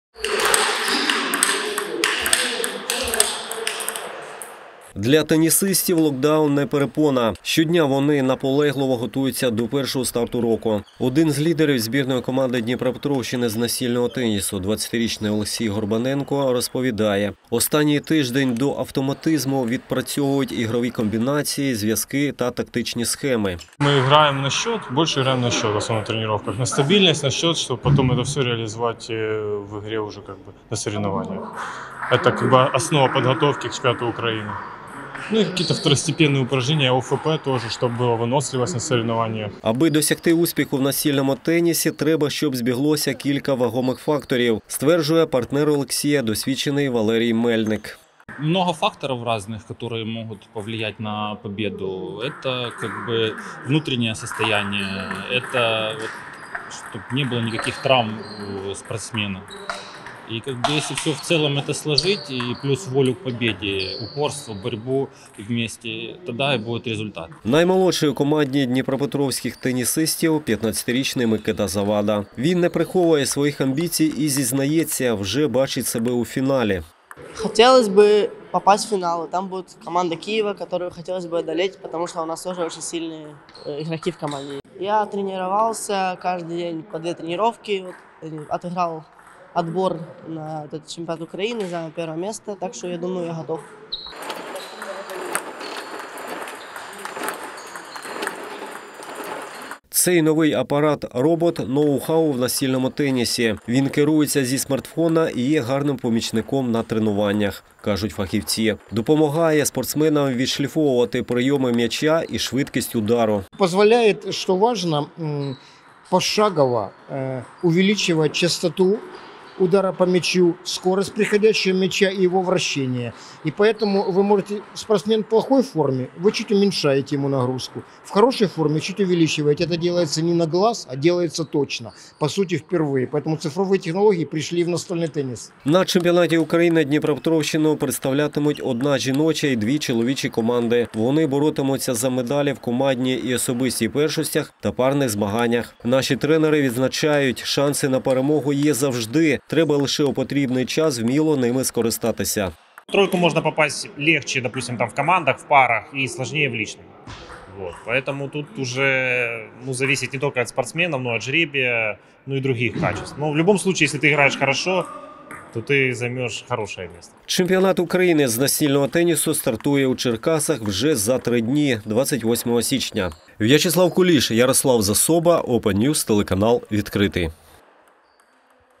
Збірна області з настільного тенісу готується до чемпіонату України
28 січня у Черкасах пройде чемпіонат України з настільного тенісу. В спортсменів збірної Дніпропетровщини тривають останні приготування.